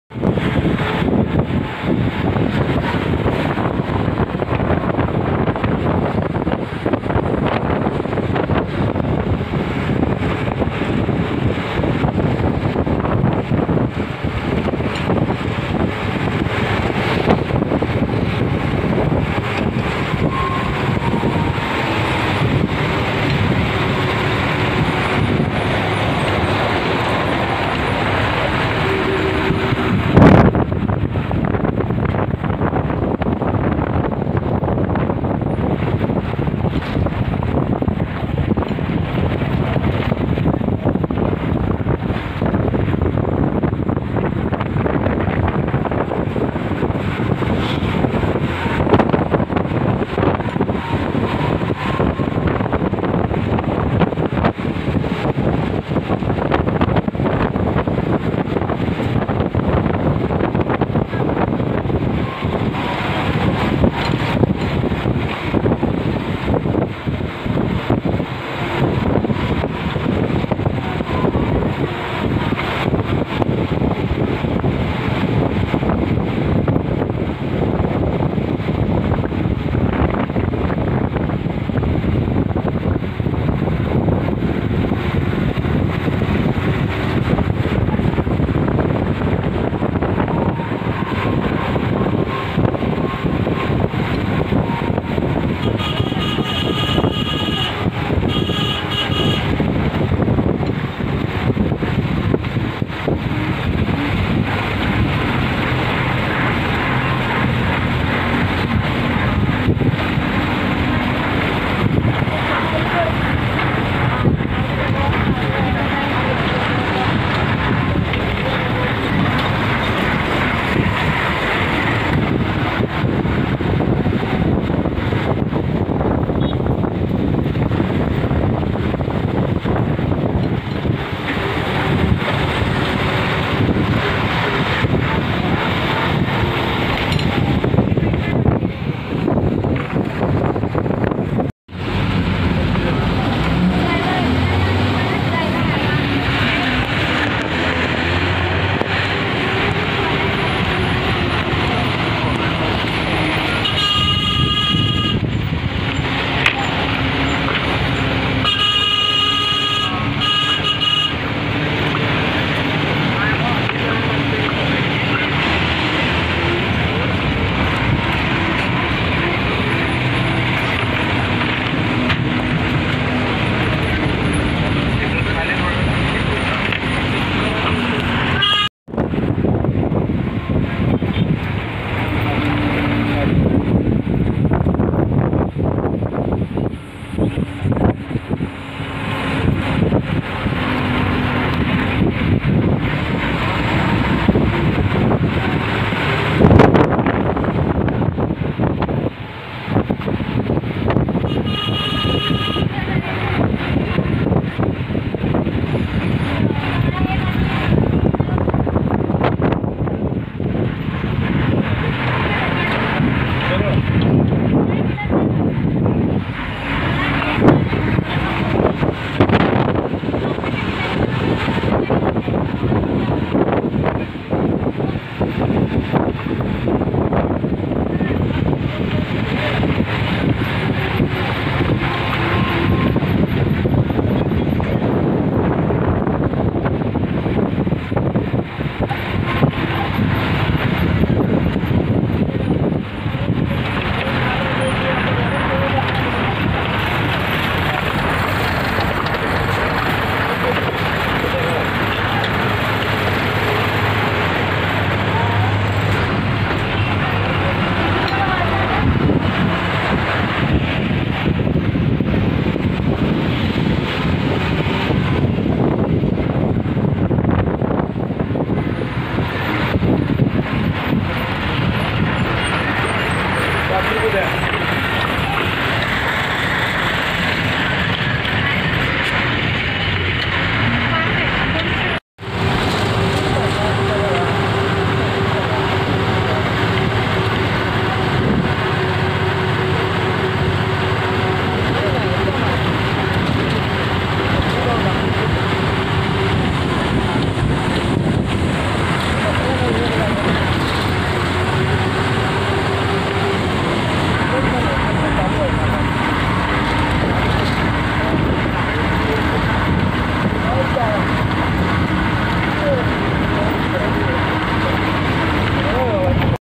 कोल्हापूर ते देवगड जाताना गगनबावडा अवघड घाट मोठी एकच गाडी जाऊ शकते
कोल्हापूर ते देवगड जाताना गगनबावडा घाट खूपच अवघड आहे एकच गाडी मोठी जाऊ शकते व्हिडिओ आवडल्यास नक्की लाईक आणि सबस्क्राईब